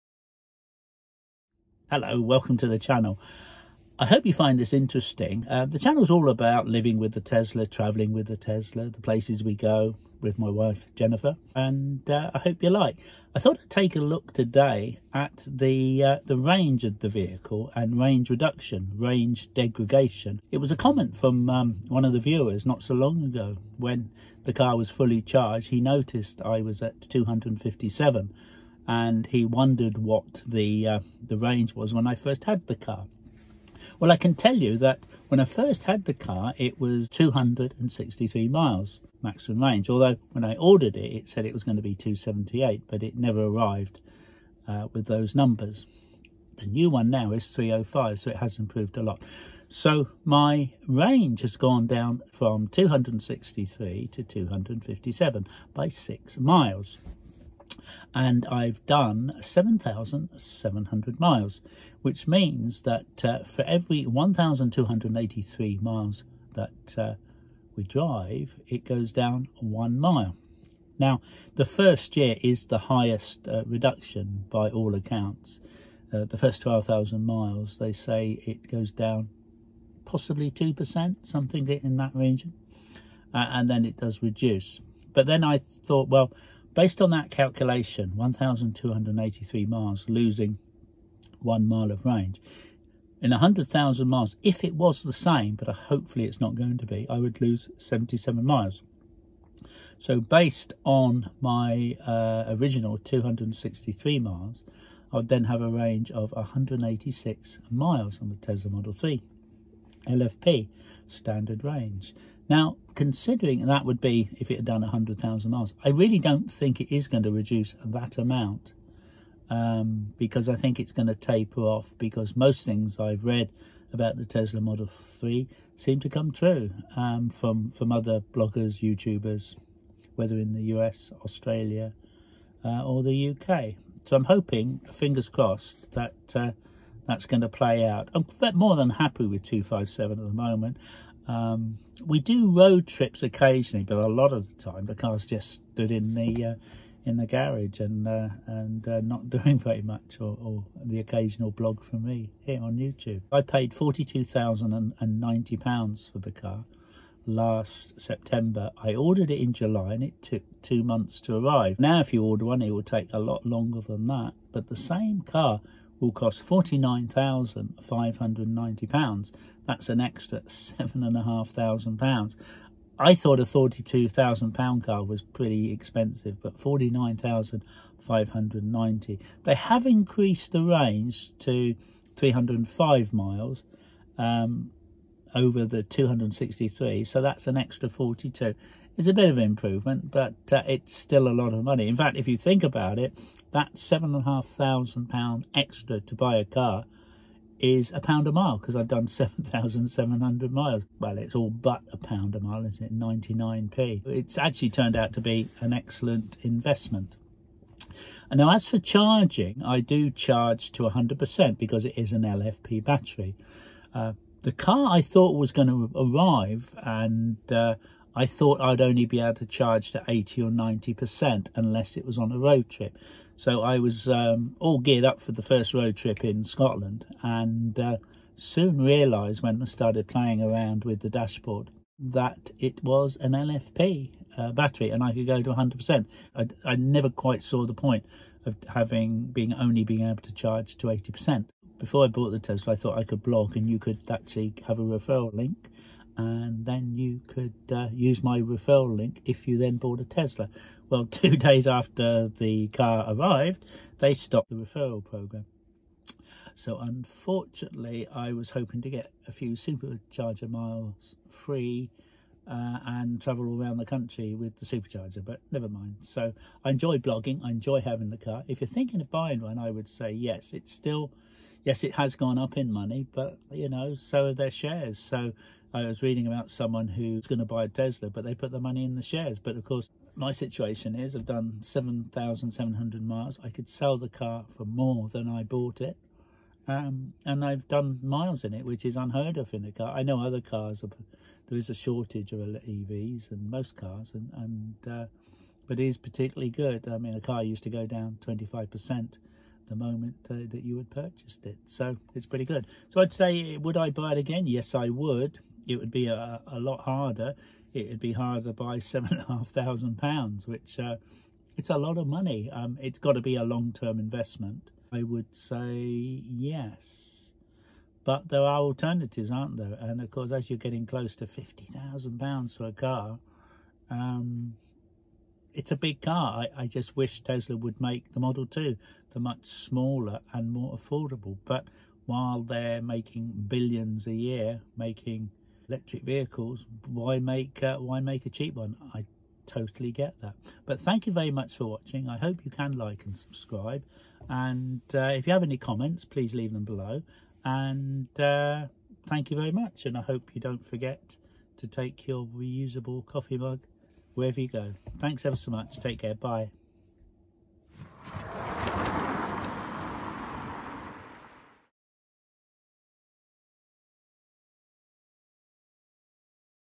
Tesla LFP True Range Degradation @Tesla Travel
How many miles our Tesla model 3 LFP battery range has reduced after 7700 miles and ten months of motoring